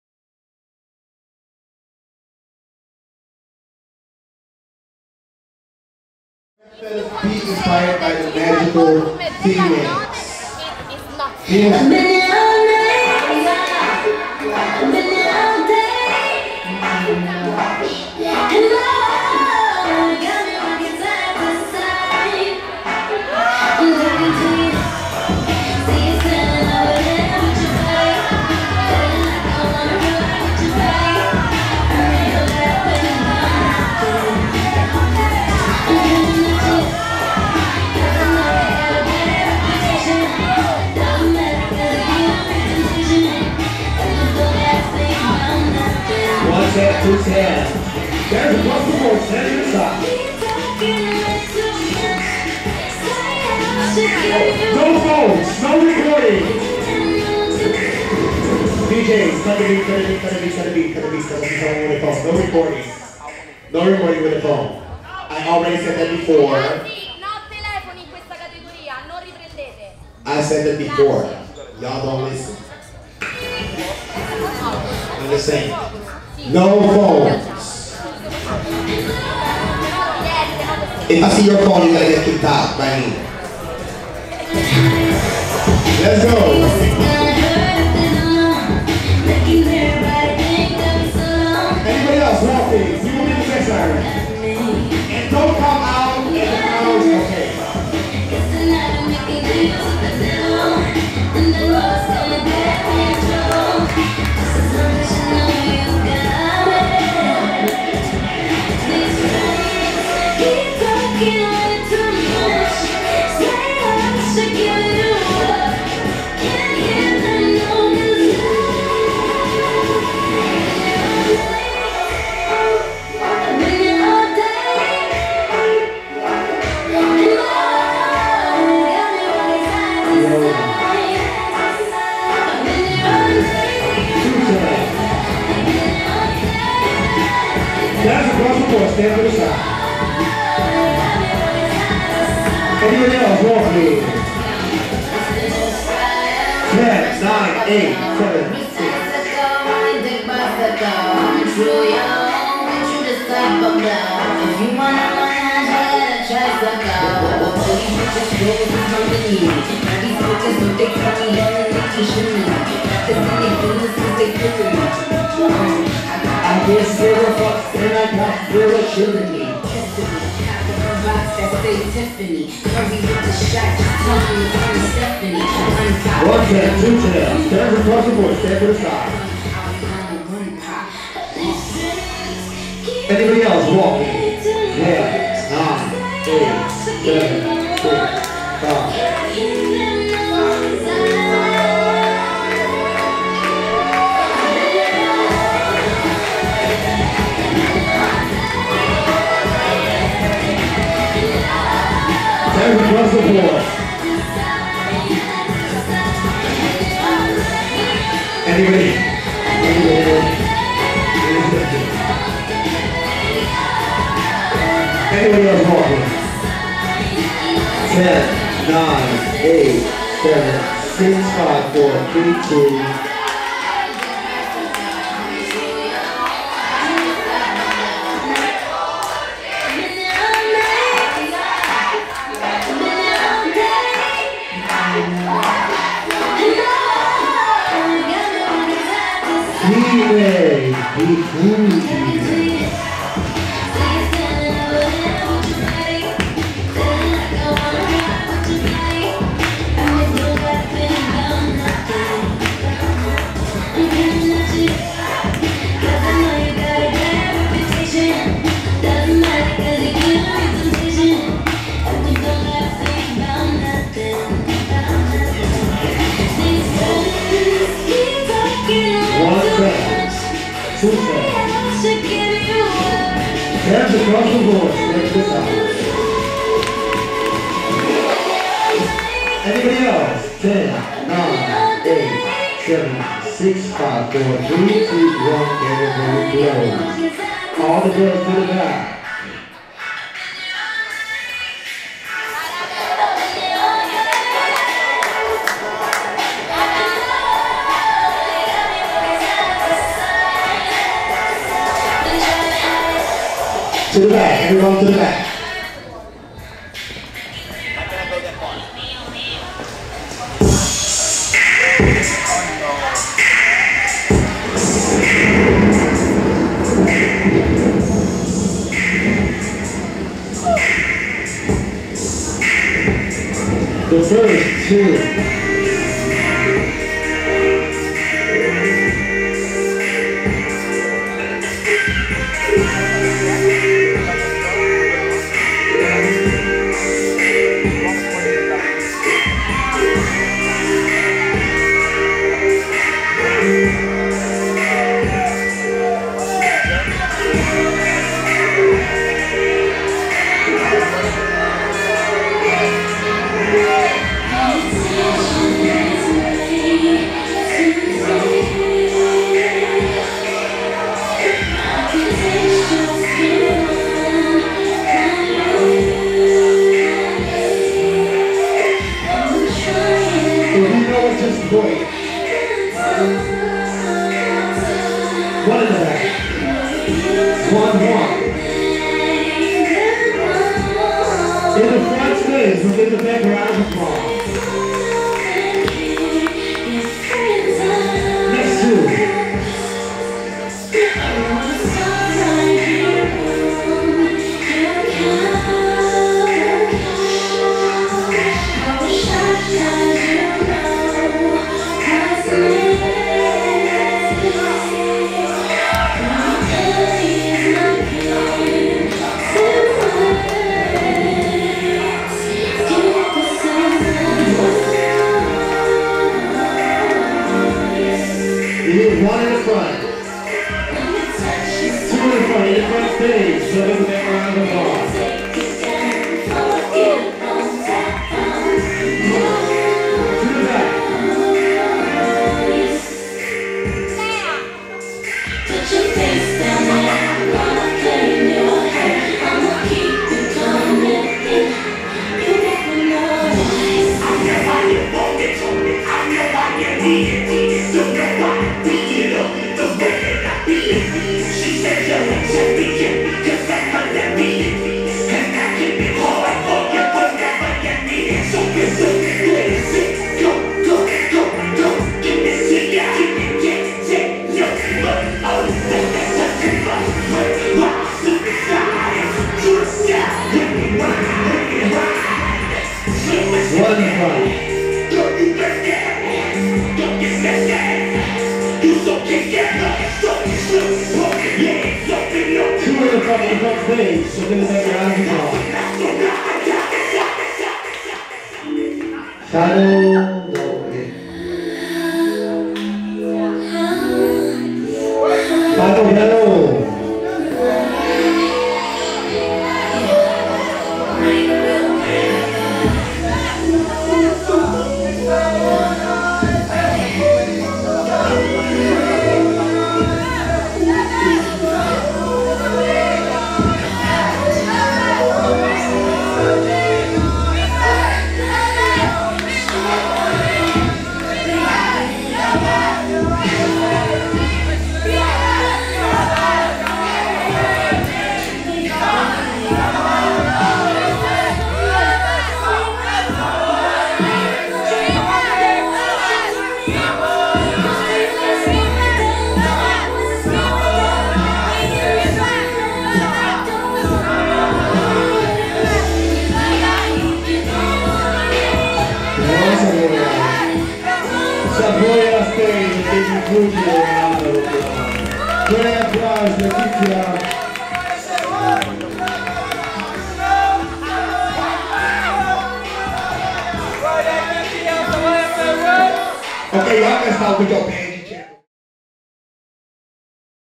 The New World Kiki Function - VogueXchange 2019 / FF Sex Siren
The New World Kiki Function - VogueXchange 2019
By Mother Anna & Father JiJi Munera
FF Sex Siren
GP Letizia Savoia